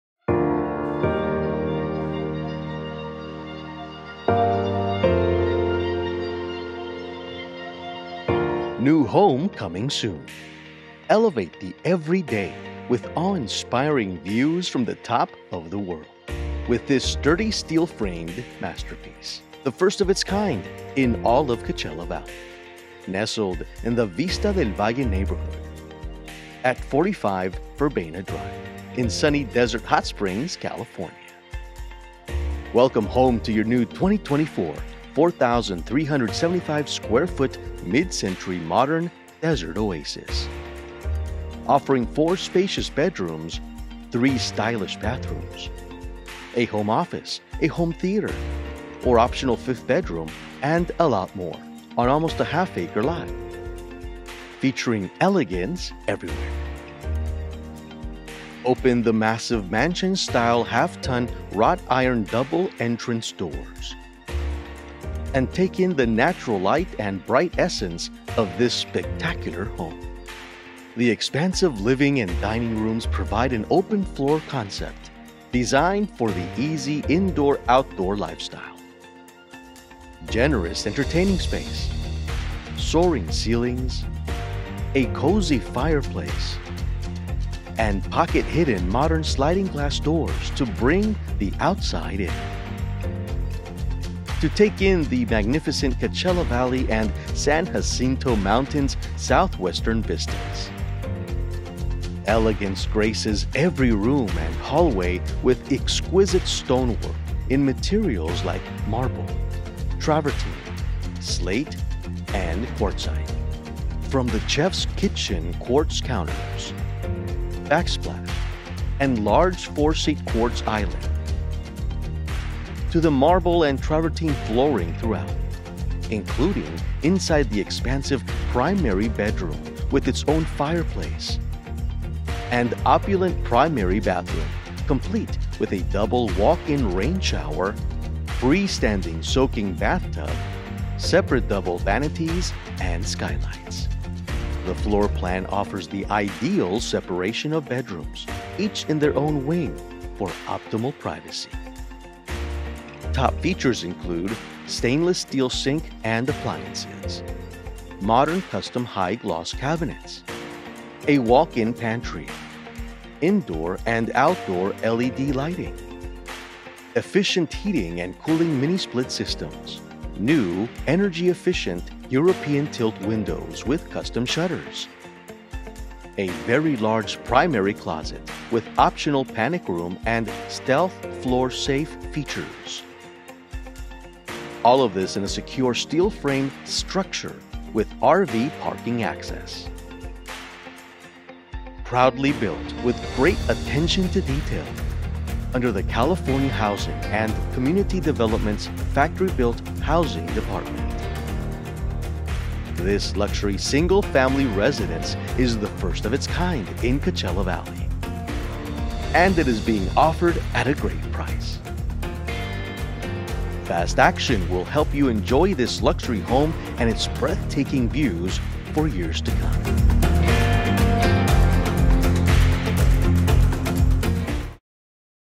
45 Verbena Dr MLS Home Tour Video | Tiny Mansions Smart Living Homes
Elevate the everyday with awe-inspiring views from the top of the world with this sturdy steel-framed masterpiece, the first-of-its-kind in all of Coachella Valley – nestled in the Vista del Valle neighborhood at 45 Verbena Dr, in sunny Desert Hot Springs, CA.

Welcome home to your new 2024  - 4,375 S.F. mid-century modern desert oasis, offering 4 spacious bedrooms, 3 stylish bathrooms, a home office, a home theater (or optional 5th bedroom) and a lot more on an almost half-acre size lot featuring elegance everywhere.

Open the massive mansion-style half-ton wrought iron double entrance doors and take in the natural light and bright essence of this spectacular home. The expansive living and dining rooms provide an open-floor concept designed for the easy indoor/outdoor lifestyle, generous entertaining space, soaring ceilings, a cozy fireplace and pocket-hidden modern sliding glass doors to bring the outside in, to take in the magnificent Coachella Valley and San Jacinto mountains southwestern vistas.

Elegance graces every room and hallway with exquisite stonework in materials like marble, travertine, slate, and quartzite. From the Chef’s kitchen quartz counters, backsplash and large 4-seat quartz island, to the marble and travertine flooring throughout, including inside the expansive primary bedroom with its own fireplace and opulent primary bathroom, complete with a double walk-in rain shower, freestanding soaking bathtub, separate double sinks and skylights.

The floor plan offers the ideal separation of bedrooms, each in their own wing, for optimal privacy.

Features include: stainless steel sink and appliances, modern custom high gloss cabinets, a walk-in pantry, indoor and outdoor LED lighting, efficient heating and cooling mini-split systems, new energy-efficient European-tilt windows with custom shutters, a large primary closet with optional panic-room and stealth floor safe features, all of this in a secure steel-frame structure with RV parking access.

Proudly built with great attention to detail by Smart Living Homes under the California Housing and Community Development’s Factory-built Housing Department, this luxury Single Family Residence is the first of its kind in Coachella Valley and it is being offered at a great price. Fast action will help you enjoy this luxury home and its breathtaking views for years to come.